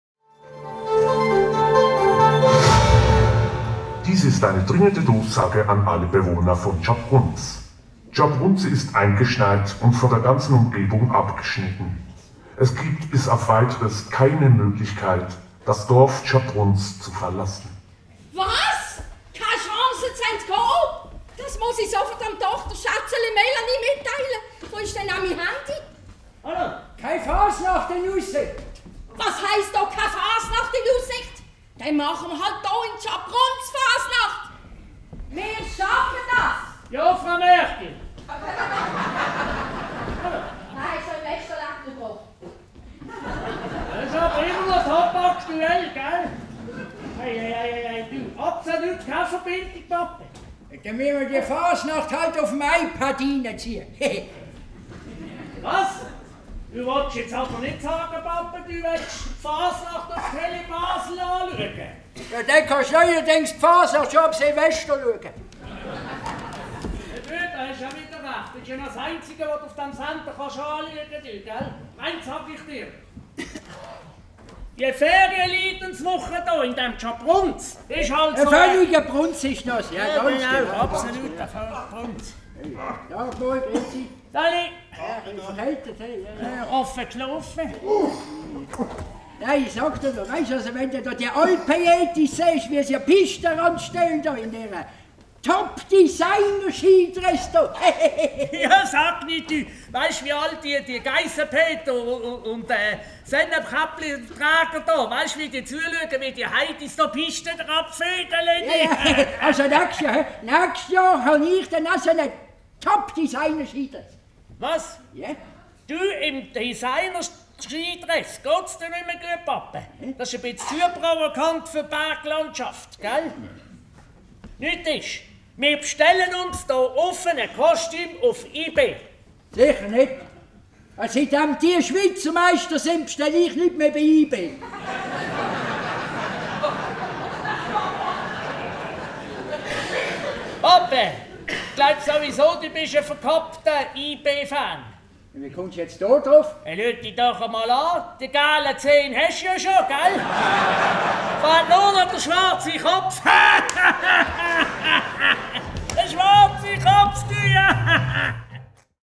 s'Läggerli 2020 : Fasnachts-Investition
s'Läggerli - Vorfasnachtsveranstaltung 2020

'Fasnachts-Investition'
Text: Aernschd Born
Aufnahme: TeleBasel